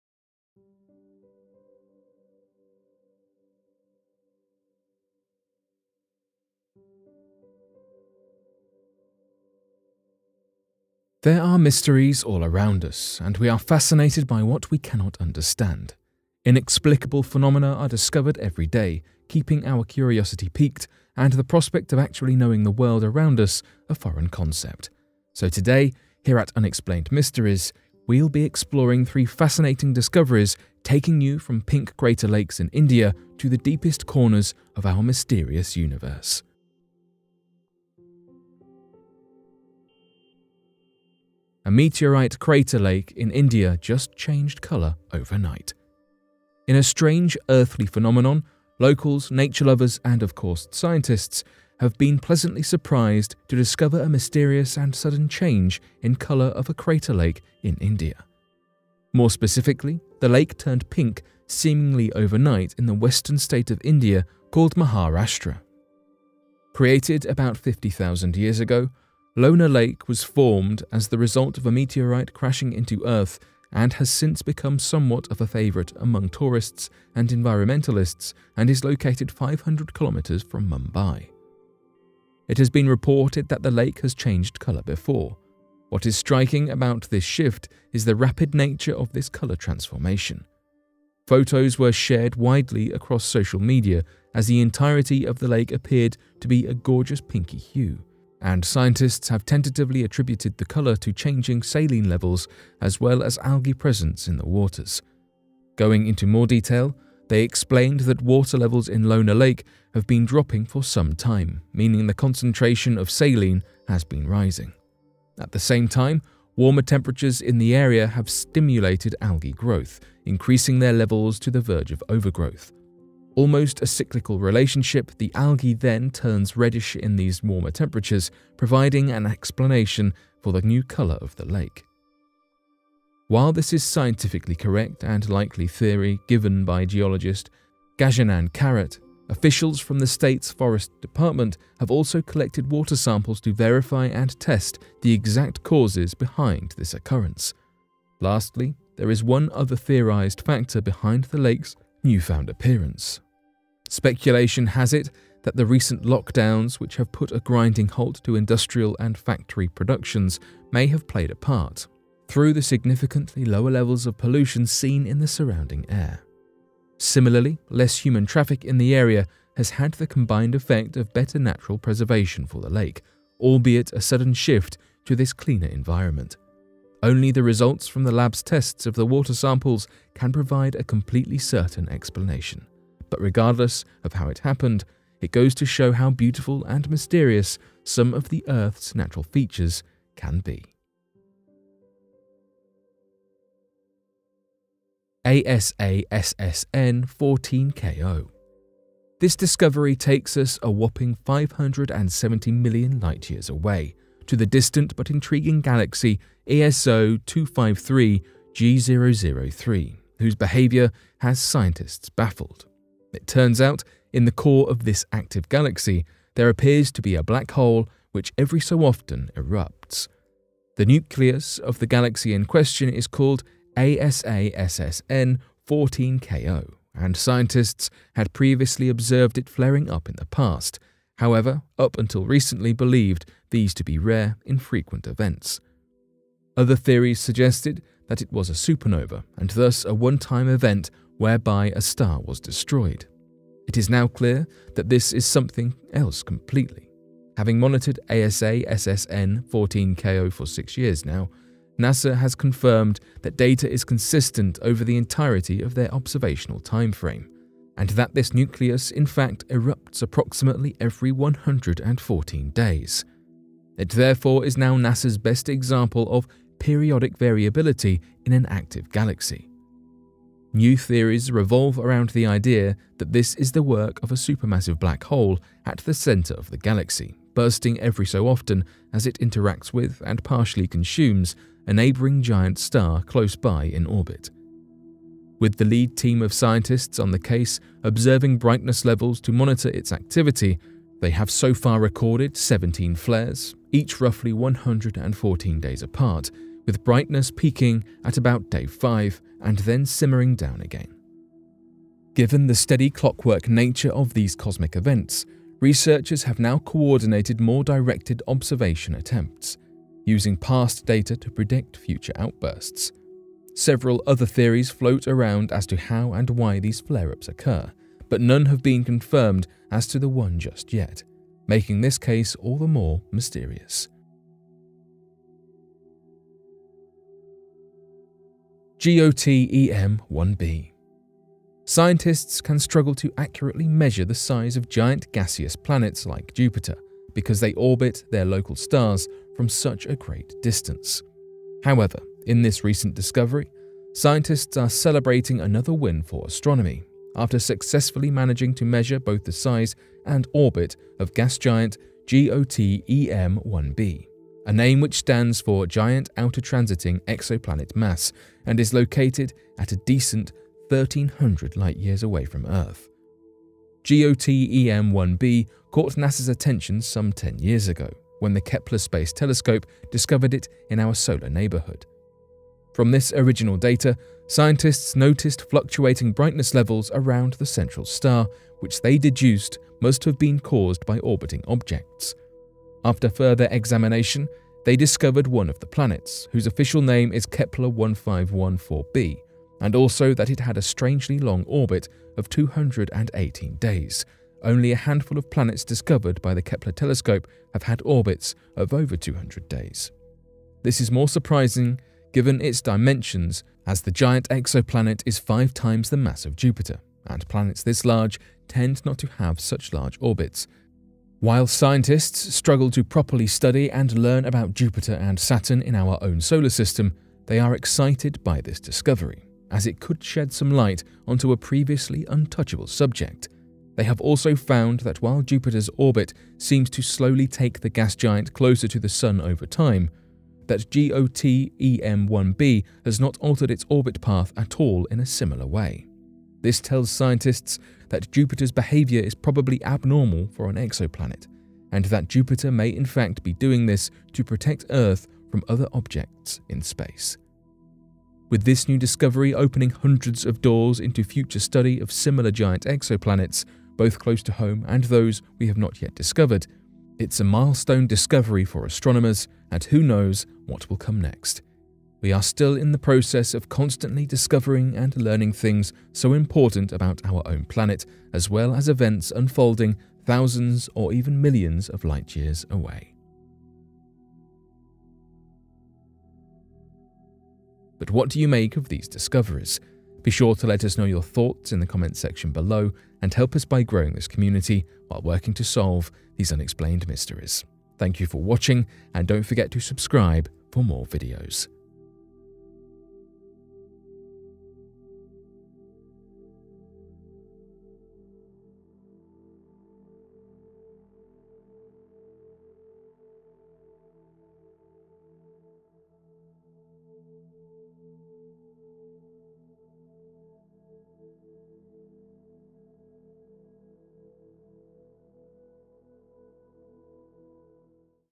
Scientists Just Announced This Huge Meteorite Crater In India Just Changed Colour Within A Few Hours
Scientists just announced this huge meteorite crater in India just changed colour within a few hours. Today, we take a look at what scientists just revealed about this interesting meteorite.

There are mysteries all around us, and we are fascinated by what we cannot understand. Inexplicable phenomena are discovered every day, keeping our curiosity piqued and the prospect of actually knowing the world around us a foreign concept.

In a strange earthly phenomenon, locals, nature-lovers and of course scientists, have been pleasantly surprised to discover a mysterious and sudden change in colour of a crater lake in India.